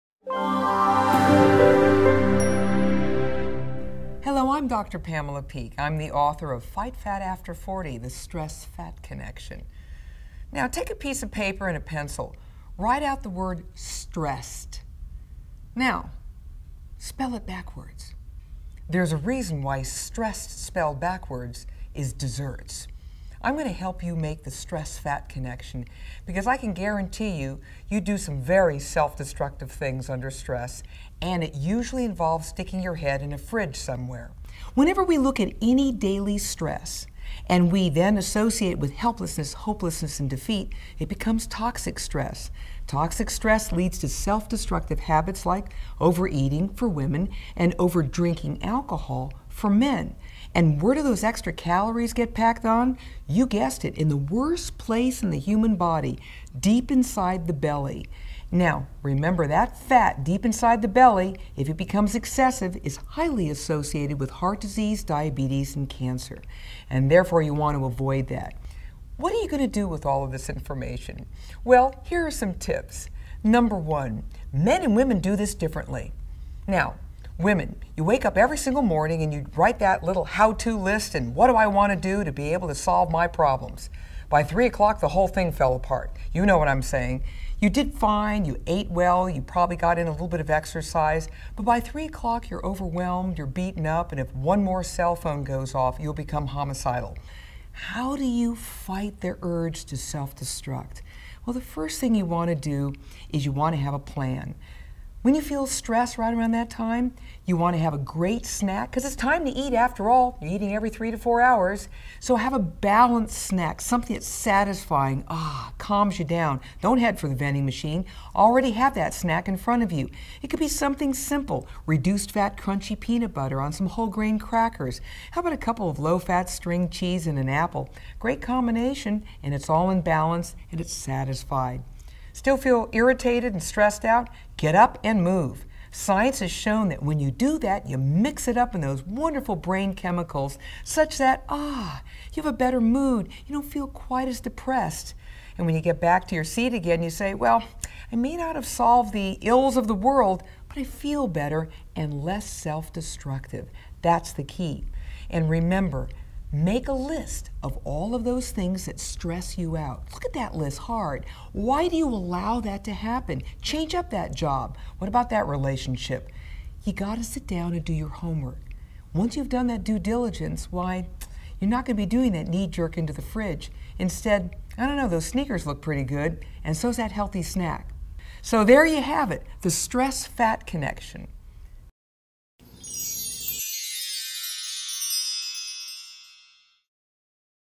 How to break the stress-fat connection
The road to stress relief should not include a rest stop at the vending machine or ice cream shop. Best-selling author Dr. Pamela Peeke has some great tips to help break the stress-fat connection that can literally be a killer.